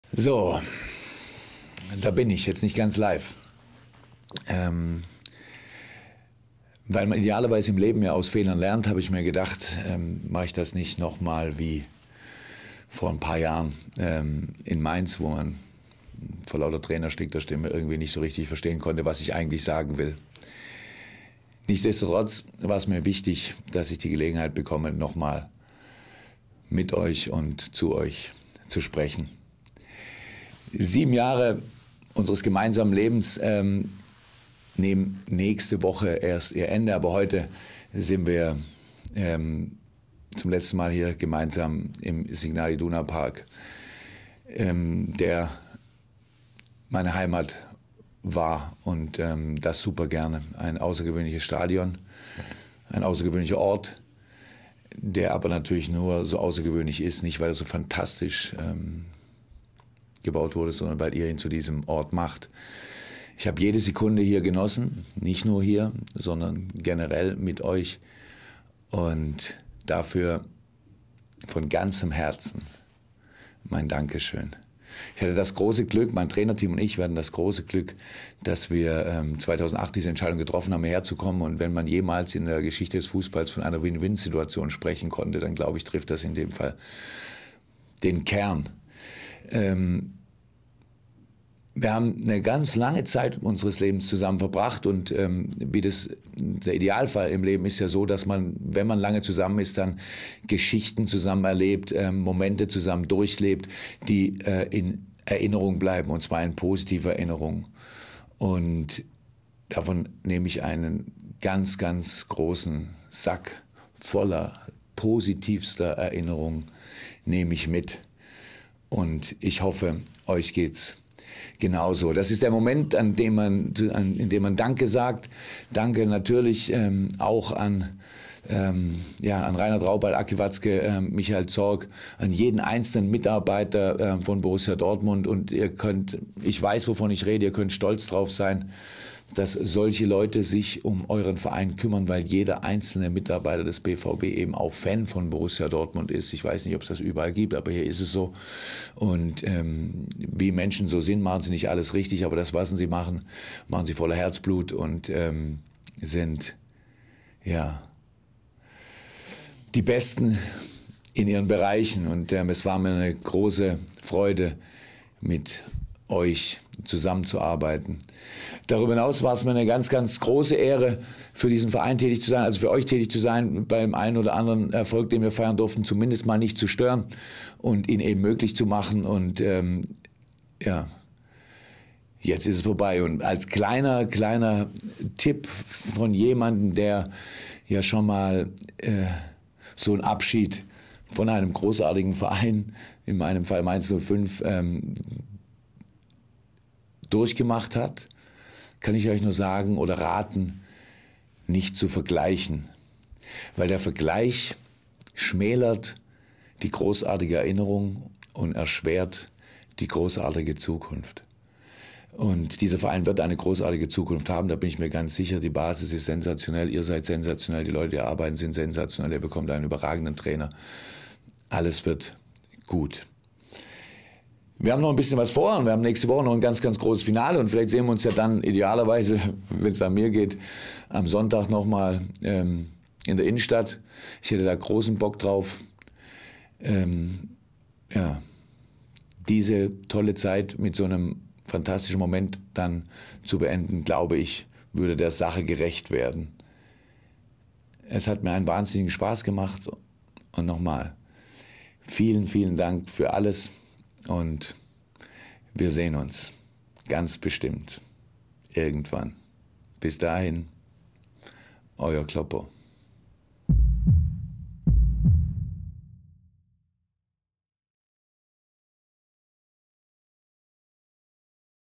A farewell message to the fans from Jürgen Klopp (English subtitles)
Jürgen Klopp's farewell message that was screened in the stadium after his final home game for Borussia Dortmund. English subtitles by bundesliga.com.